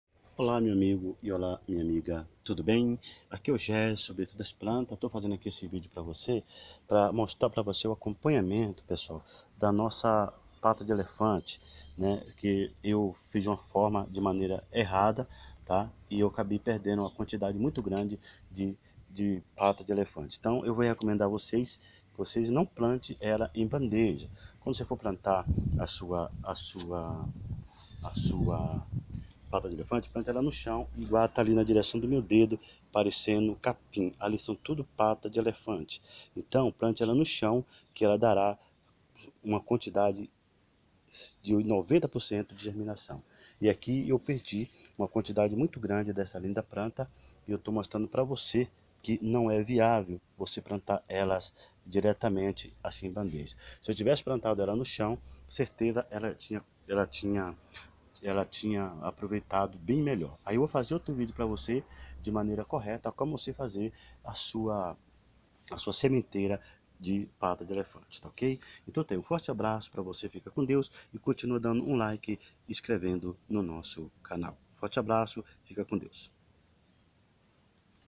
MUDAS DE PATAS DE ELEFANTE RECÉM GERMINADAS
Uma dica: NUNCA COLOQUE SEMENTES DE PATAS DE ELEFANTE PARA GERMINAR EM BANDEJA ,OU OUTROS,MAS SIM DIRETAMENTE NO SOLO,POR QUE PEGA A ÚMIDA PADRÃO.